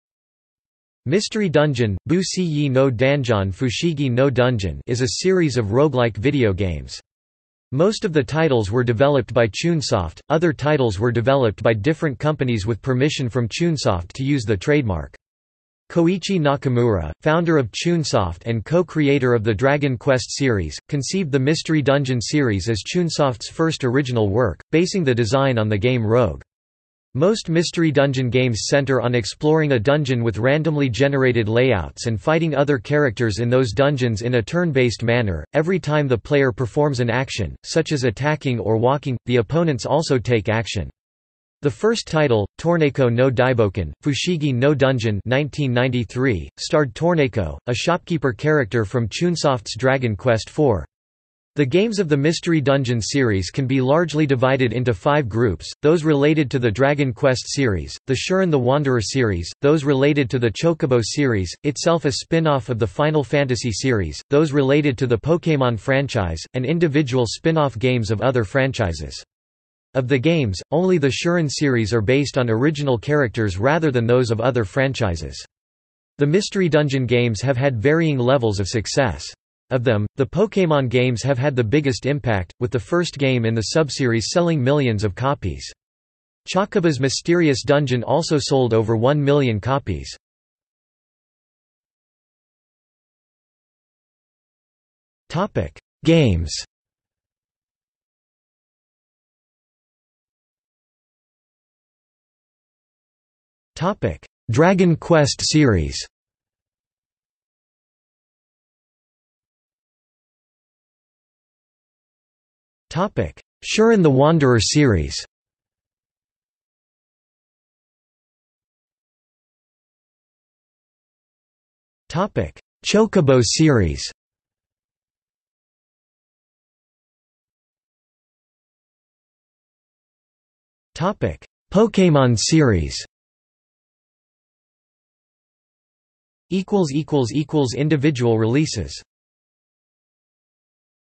List of Mystery Dungeon video games | Wikipedia audio article
00:01:31 1 Games
00:01:40 1.1 iDragon Quest/i series
00:01:49 1.2 iShiren the Wanderer/i series
00:01:58 1.3 iChocobo/i series
00:02:07 1.4 iPokémon/i series

- Socrates

SUMMARY
Mystery Dungeon (不思議のダンジョン, Fushigi no Dungeon) is a series of roguelike video games. Most of the titles were developed by Chunsoft; other titles were developed by different companies with permission from Chunsoft to use the trademark. Koichi Nakamura, founder of Chunsoft and co-creator of the Dragon Quest series, conceived the Mystery Dungeon series as Chunsoft's first original work, basing the design on the game Rogue. Most Mystery Dungeon games center on exploring a dungeon with randomly generated layouts and fighting other characters in those dungeons in a turn-based manner; every time the player performs an action, such as attacking or walking, the opponents also take action. The first title, Torneko no Daibōken: Fushigi no Dungeon (1993), starred Torneko, a shopkeeper character from Chunsoft's Dragon Quest IV. The games of the Mystery Dungeon series can be largely divided into five groups: those related to the Dragon Quest series; the Shiren the Wanderer series; those related to the Chocobo series, itself a spinoff of the Final Fantasy series; those related to the Pokémon franchise; and individual spin-off games of other franchises. Of the games, only the Shiren series are based on original characters rather than those of other franchises. The Mystery Dungeon games have had varying levels of success. Of them, the Pokémon games have had the biggest impact, with the first game in the subseries selling millions of copies. Chocobo's Mysterious Dungeon also sold over one million copies.